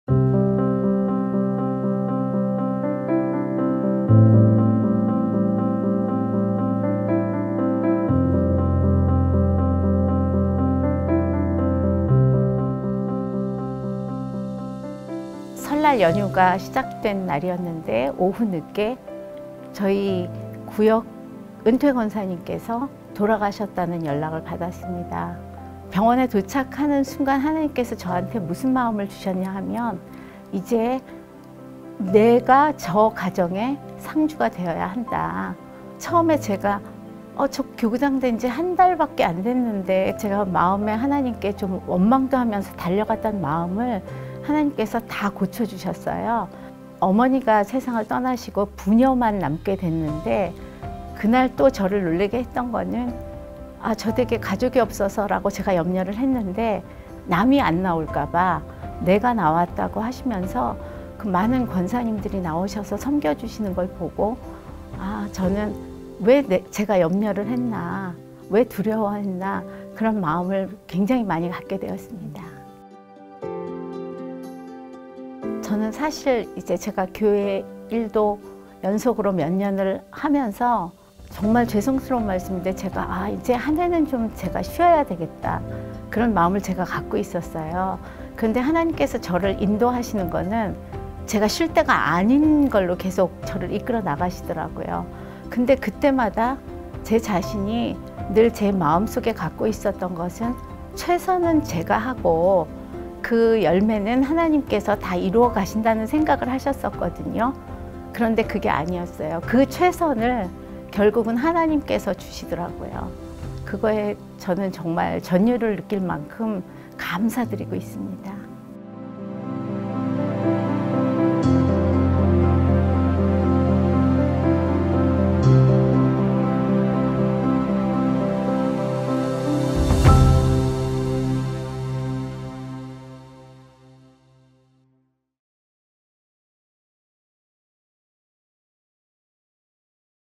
[소망교회] 구역 이야기 / 문정1교구 김현정 권사 / 소망이야기
2020년 상반기, 구역원과 구역지도자들에게
구역에 대한 이야기를 들어보았습니다.